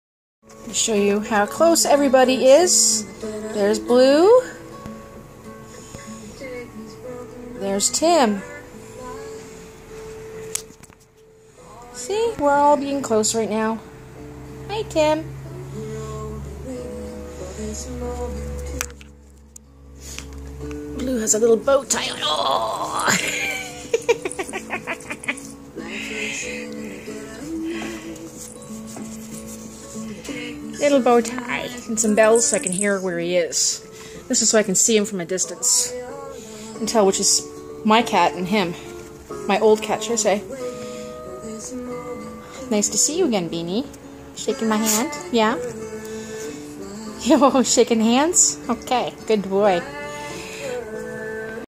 Mishka (Blue) and Beanie sitting with Tymone near
Introduction of 2 cats working